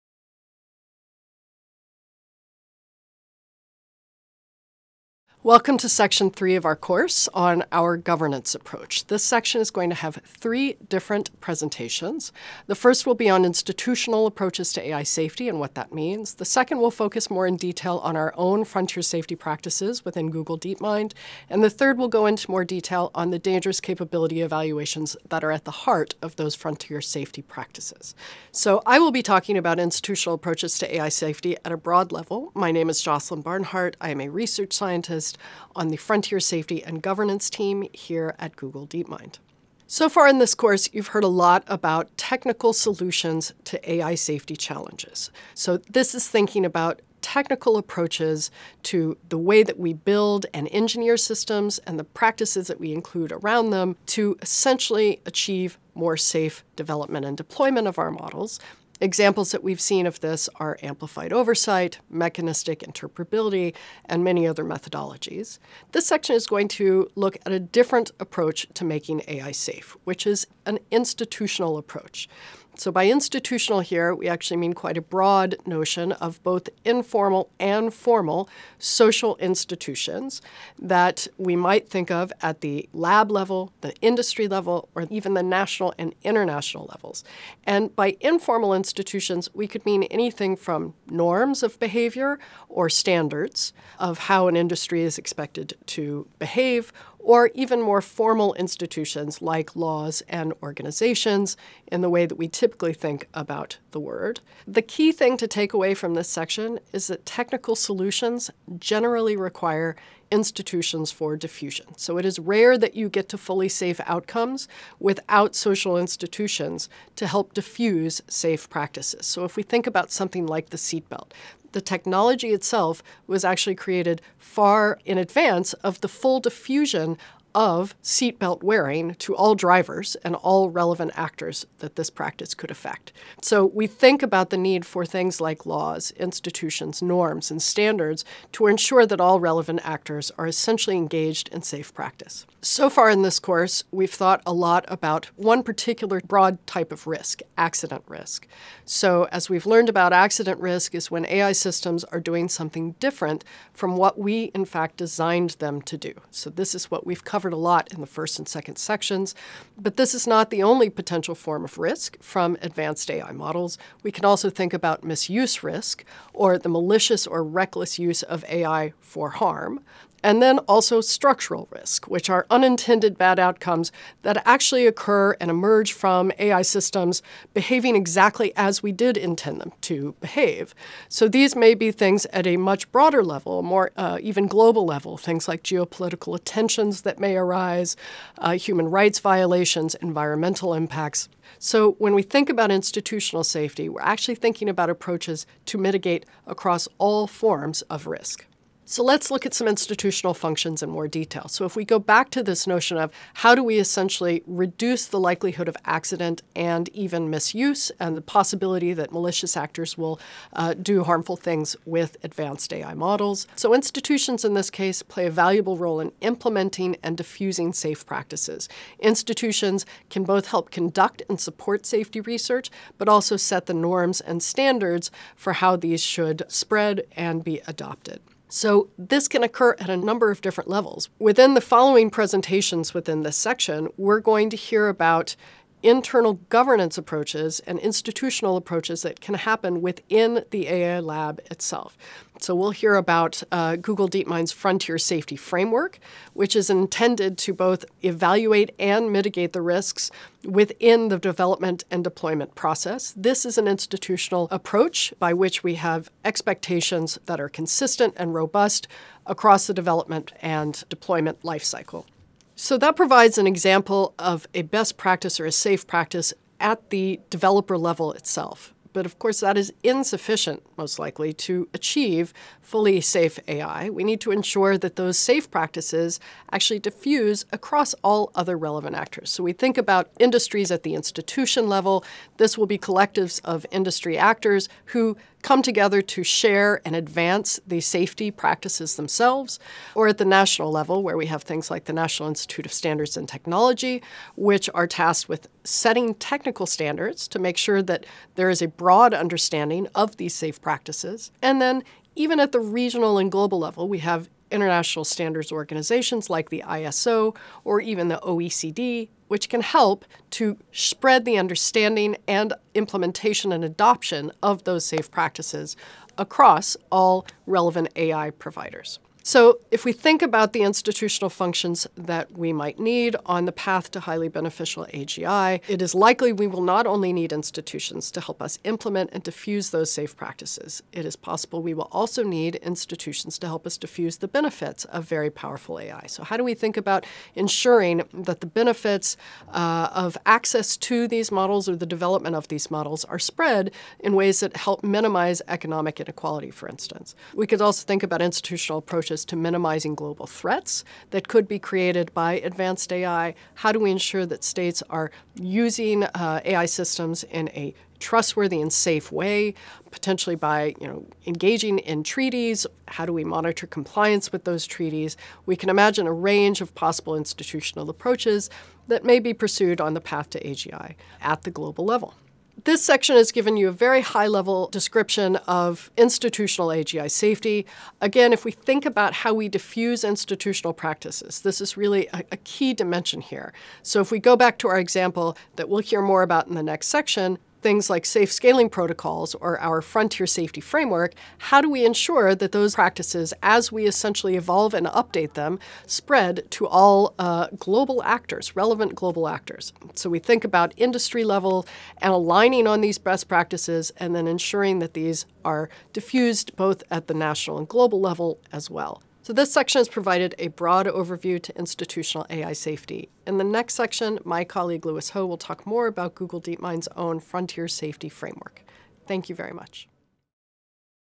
Part 3: 1. Institutional approaches to AI Safety
Joslyn Barnhart introduces the final part of the AGI safety course focusing on AI governance. She discusses the challenges and strategies for effectively governing increasingly powerful AI systems.